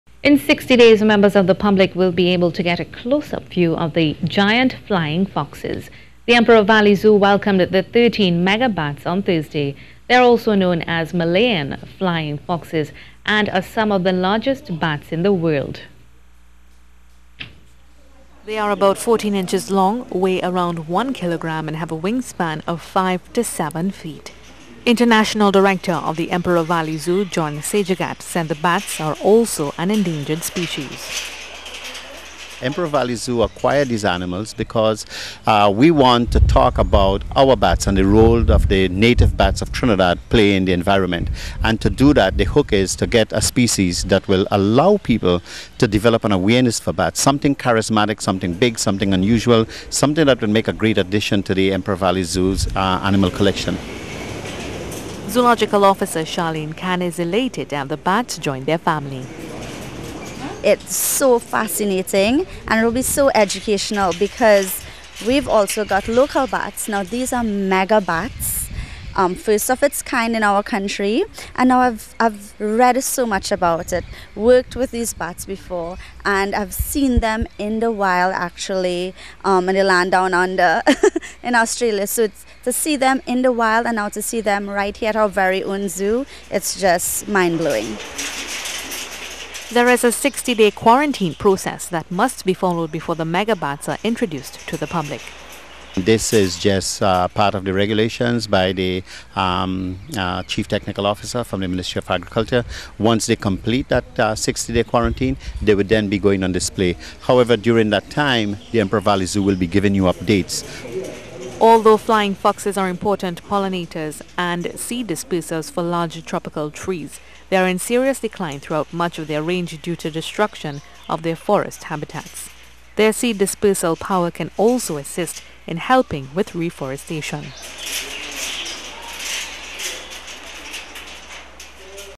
Emperor Valley Zoo Welcomes 13 Malayan Flying Foxes
In 60 days, members of the public will be able to get a close-up view of the Giant Flying Foxes. The Emperor Valley Zoo welcomed the thirteen Megabats on Thursday.

They're also known as Malayan flying foxes and are some of the largest bats in the world.